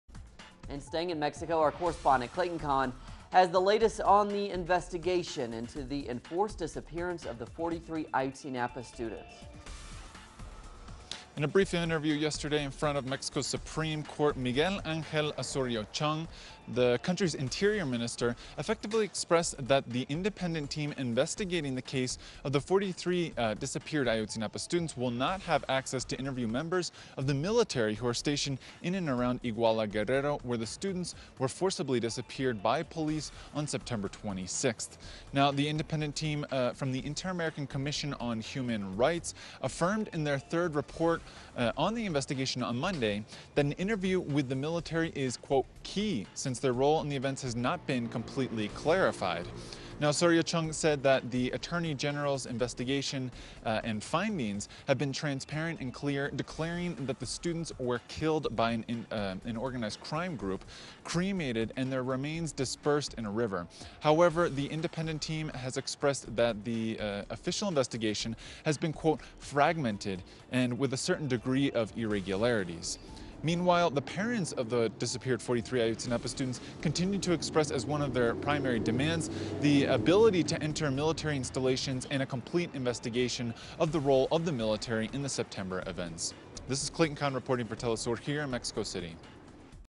Mexico: Investigators Barred from Interviewing Military in Ayotzinapa
In a brief statement on Tuesday,  Mexico’s Interior Minister Miguel Angel Osorio Chong said that the independent team investigating the case of the 43 disappeared Ayotzinapa students will not be able to interview military officials stationed in and around Iguala, Guerrero, where the students were forcibly disappeared on September 26. The team from the Inter-American Commission on Human Rights said in its report that an interview with the military is “key,” since their role in the events has not been completely clarified. Osorio Chong said that the Mexico Attorney General’s investigation and findings have been transparent and clear, verifying that the students were kidnapped by an organized crime group, cremated, and their remains dispersed in a river. The independent team, however, has said that the official investigation is “fragmented,”  and plagued with a “certain degree of irregularities.” The parents of the disappeared students continue to demand entrance into military installations as part of a complete investigation of the role of the military in the September events. Clayton Conn reports from Mexico City for teleSUR.